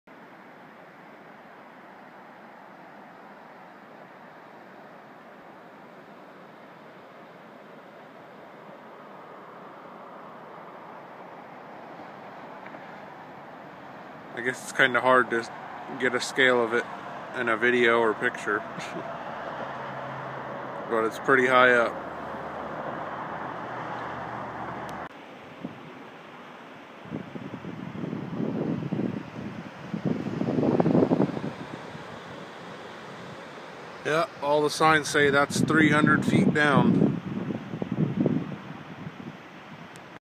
Crooked River Bridge US97
Crooked River (High) Bridge on US97 a few miles north of Redmond Oregon